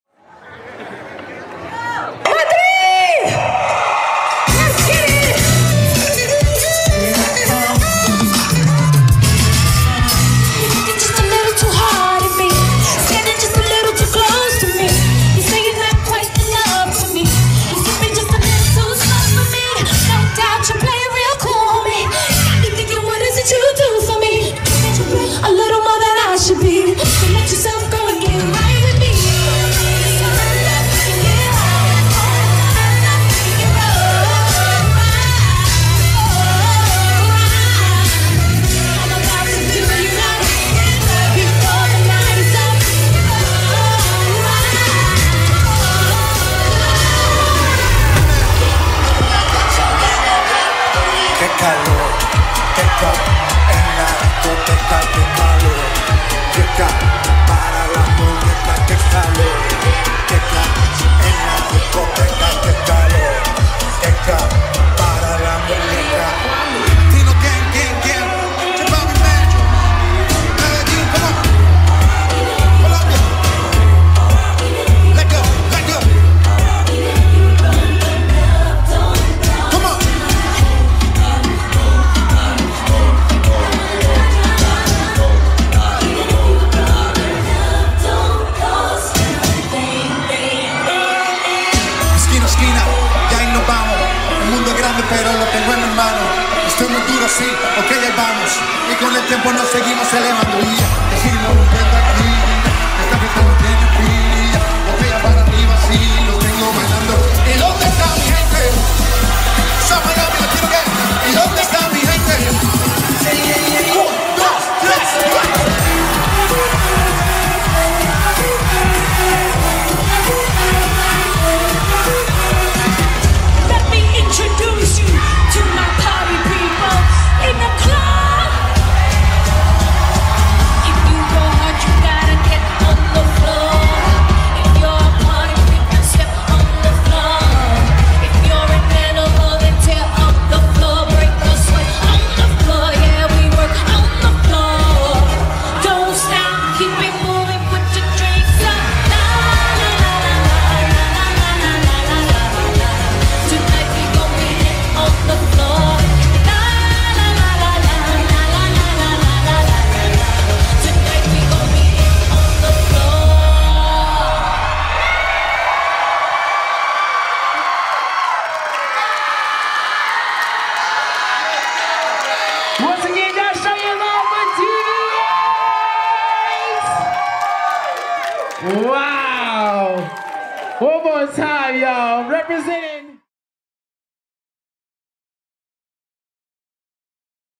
DV8 Dance Company | Maxt Out 2022 [@VIBRVNCY Front Row 4K]
Exhibition | DV8 Dance Company
Media Coverage by @VIBRVNCY
Filmed by John Kim @hellojohnkim
Maxt Out 2022 presented by Dance Your Life Podcast
Alhambra Park Bandshell 11.20.22